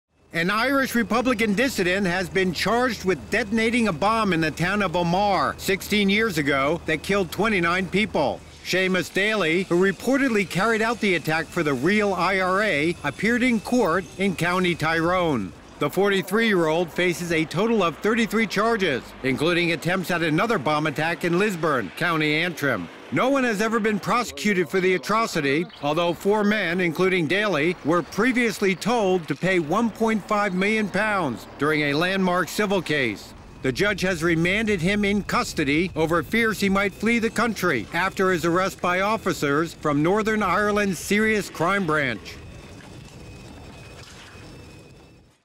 Irish Republican Dissident Bomber Charged: Seamus Daly accused of deadly Omagh bombing
An Irish republican dissident has been charged with detonating a bomb in the town on Omagh sixteen years ago that killed 29 people. Seamus Daly, who reportedly carried out the attack for the real IRA, appeared in court in Country Tyrone. The 43-year-old faces a total of 33 charges, including attempts at another bomb attack in Lisburn, County Antrim. No-one has ever been prosecuted for the atrocity although four men, including Daly, were previously told to pay 1.5 million pounds during a landmark civil case. The judge has remanded him in custody over fears he might flee the country after his arrest by officers from Northern Ireland's Serious Crime Branch.